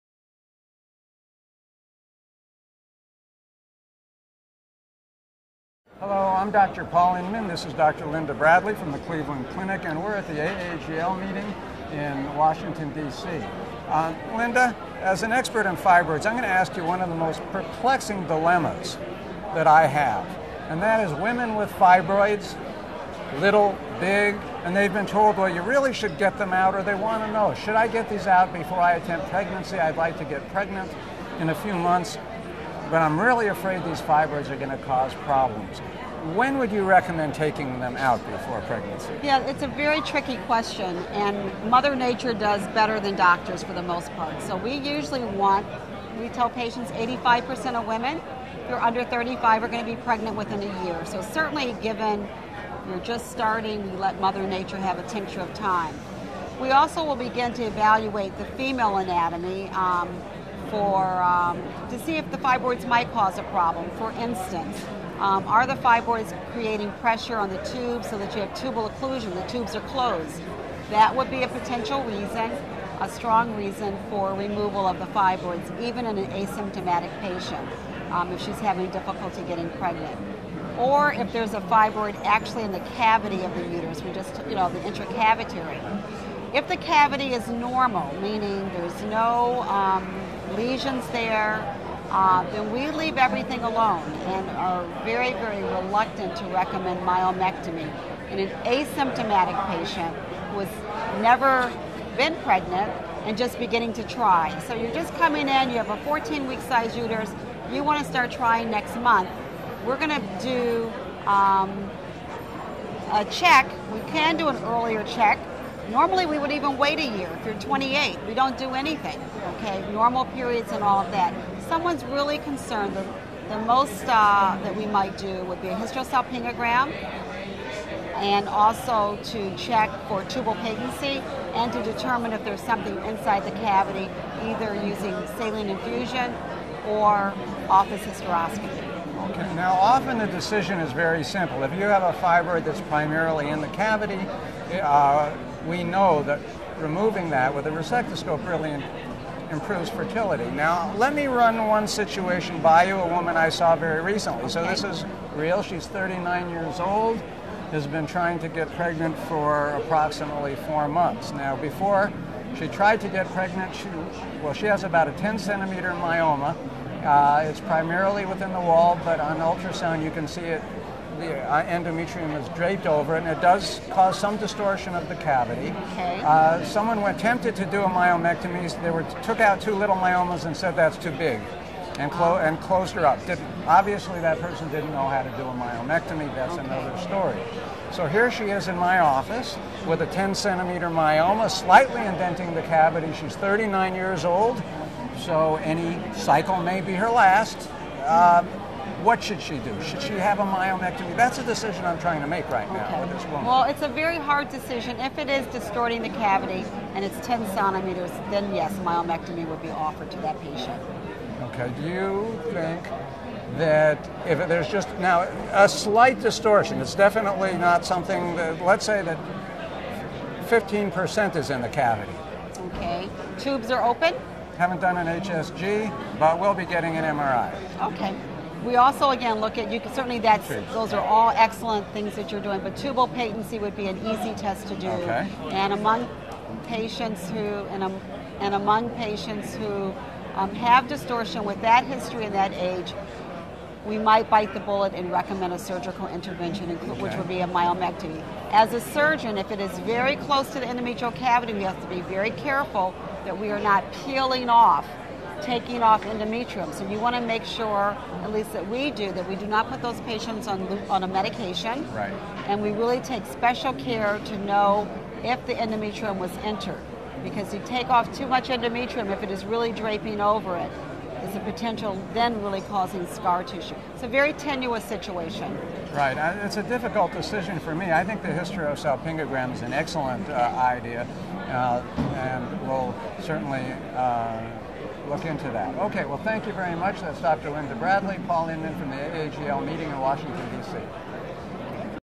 Fibroids and Contraception
Paul Indman, MD, and Linda Bradley, MD discuss fibroids and contraception, at the AAGL 2007 meeting in Washington DC.